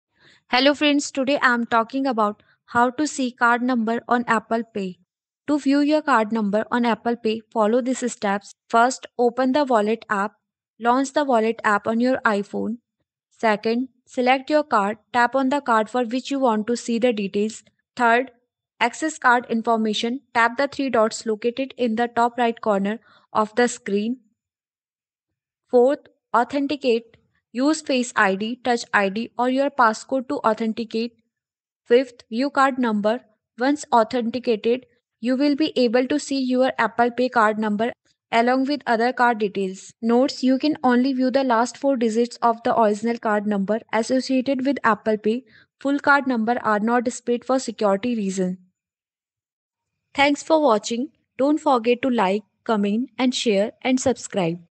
How To See Card Number On Apple Pay
In This Informative Video, We Will Guide You Through The Process Of Viewing Your Card Number On Apple Pay. Whether You Need To Reference Your Card Details For Online Purchases Or Manage Your Payment Methods, This Tutorial Will Provide Step-By-Step Instructions To Help You Access Your Card Information Securely.